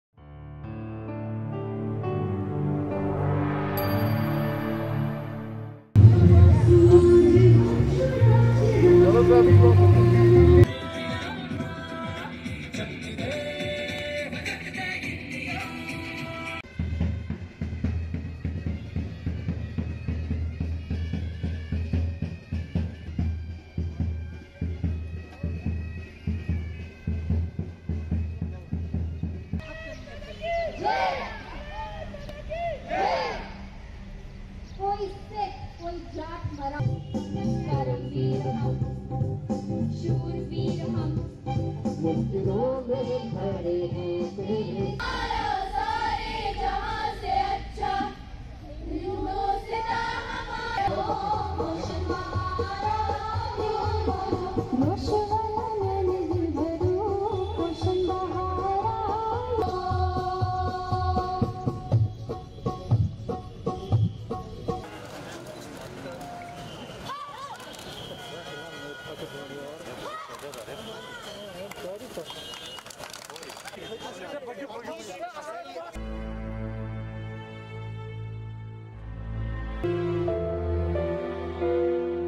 Army war veterans along with Locals and army carry the Victory Flame during the Tiranga rally
Army war veterans along with Locals and army soldiers carry the Victory Flame during the Tiranga rally organised to celebrate the victory of India in the Indo Pak War 1971, in Srinagar. The Victory Flame which was lit by the Prime Minister Narendra Modi on 16th December 2020, reached Srinagar.